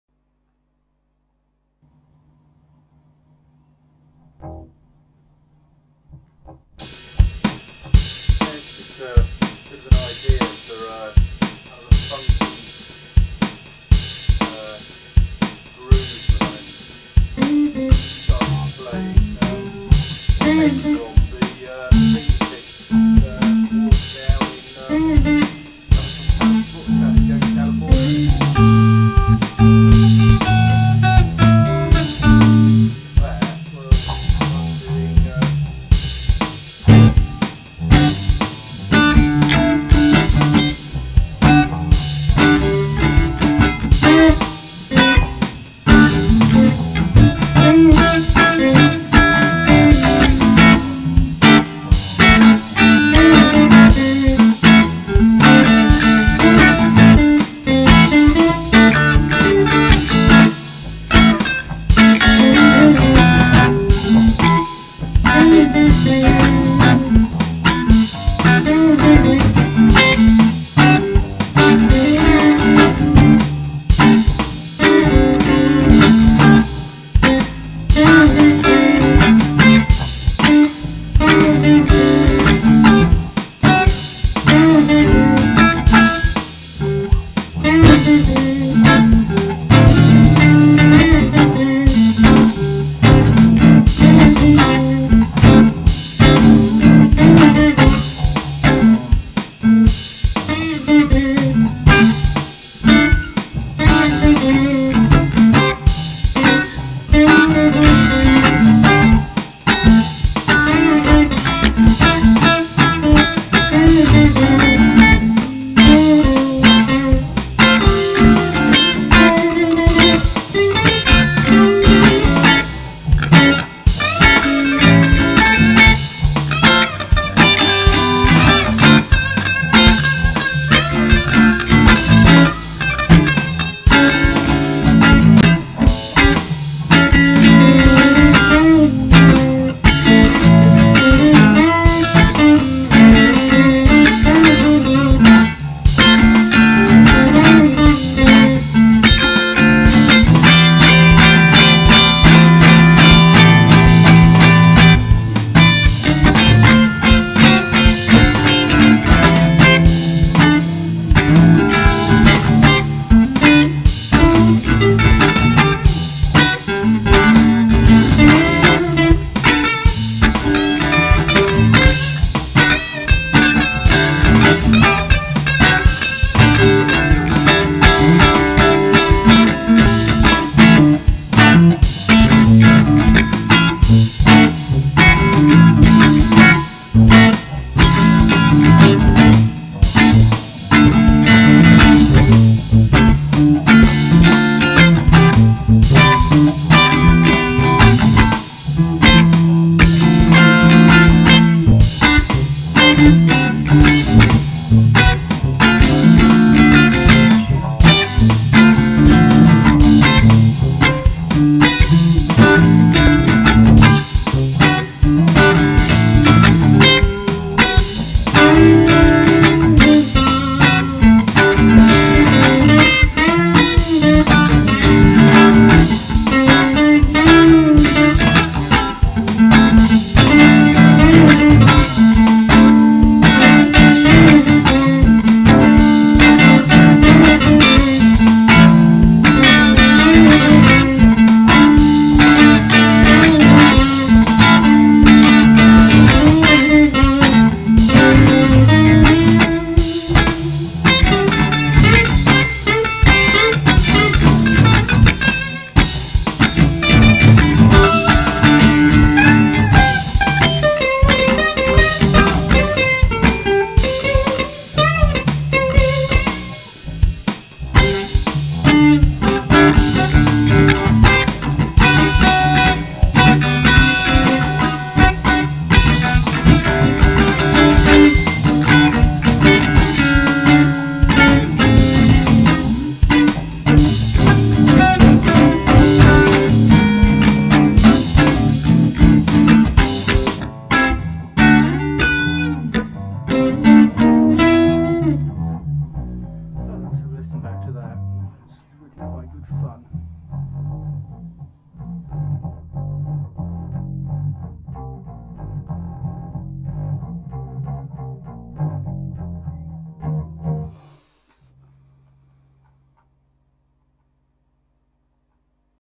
Funk idea
Developing a funk groove. Rythym Lead and Bass. Sketches from the woodshed.